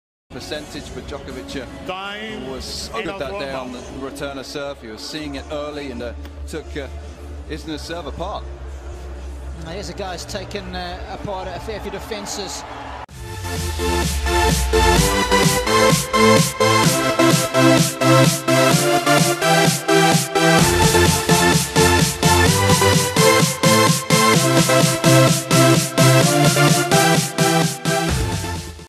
Cristiano Ronaldo SAVE a Tennis Ball From Hitting his Wife Georgina
OMG
What a Save by CR7